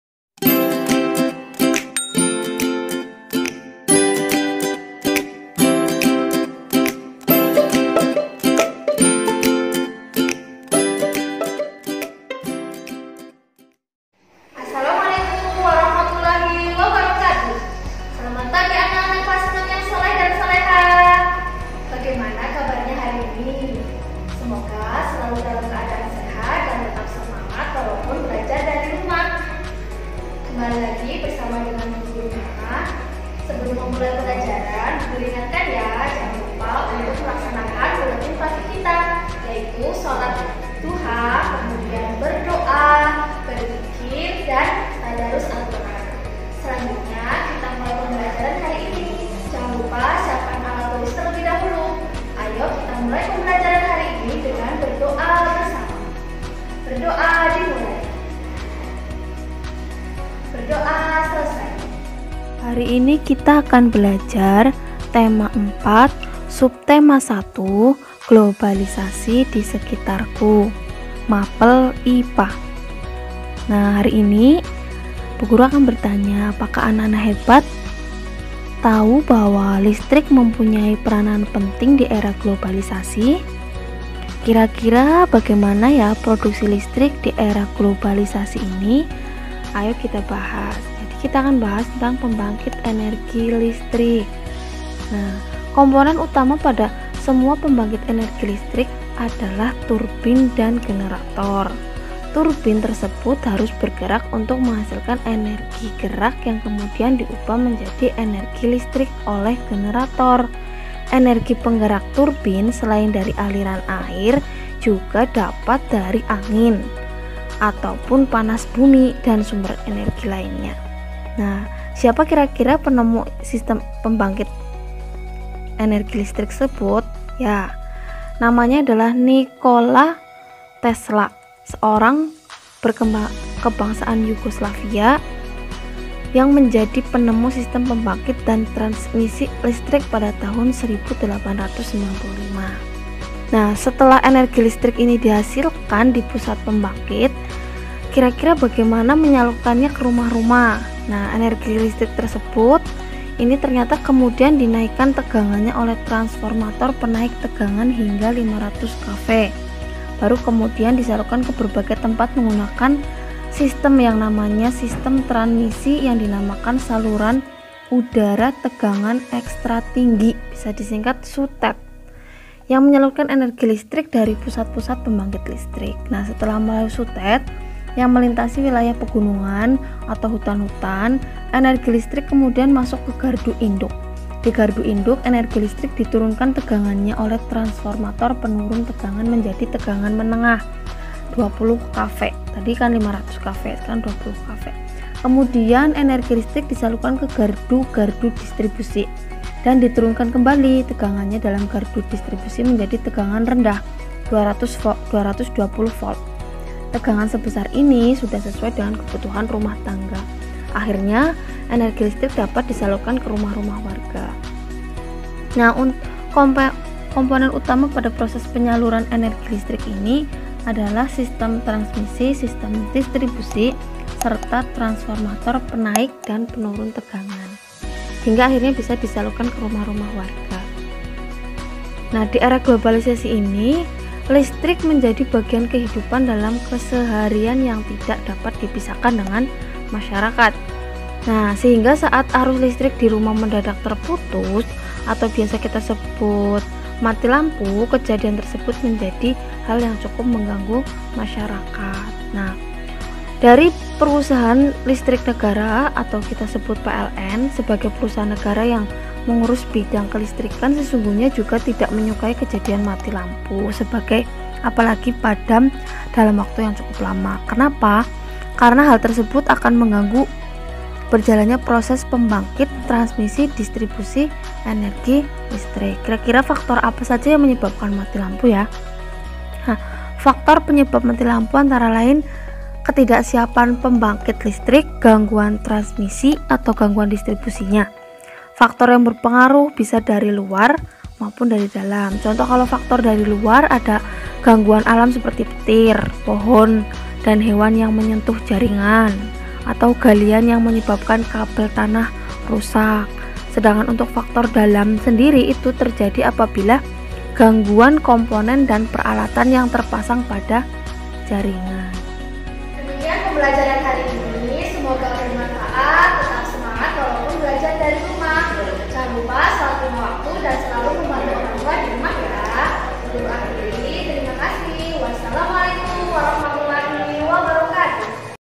VIDEO PEMBELAJARAN KELAS 6 - TEMA 4 SUBTEMA 1 - IPA
Berisi materi pembelajaran kelas 6 Tema 4 Subtema 1 Globalisasi di sekitarku - bersama dengan Bu Nikmah SD Muhammadiyah Cipete